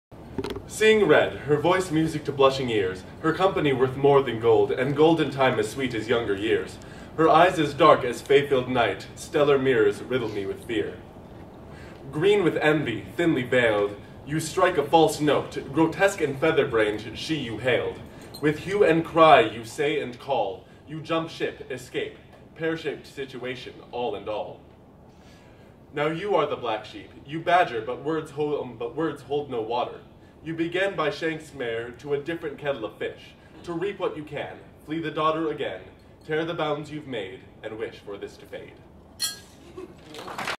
Word Salad
Word Salad is a day-of-event challenge where the challengees are provided with twenty-two words or short phrases (available the day of the event from the Provost), incorporate all of them -- nicely dressed and garnished -- into a poem, song, or other short work. Gregor Tarrachson was up to the challenge at Bardic Madness XXII in the Shire of Rokeclif (La Crosse, WI) on March 24, 2012.